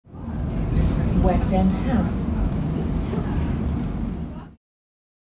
West End House
iBus announcement recorded on route 251 (VMSL2843) on the 12/01/2024